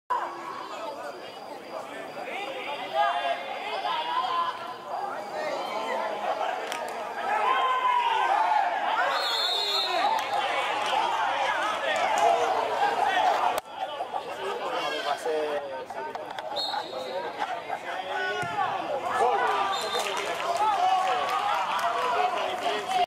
Espino (Sporting San José) marca de penalti (2-1)
Gol de Espino (Sporting San José), de penalti, minuto 63 (2-1). Partido de fútbol CD Maspalomas-Real Sporting San José. Regional Preferente Las Palmas. Jornada 7. 7/10/2016. Temporada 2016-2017. Campo de Fútbol Municipal Maspalomas 1. San Bartolomé de Tirajana. Resultado final: 4-1.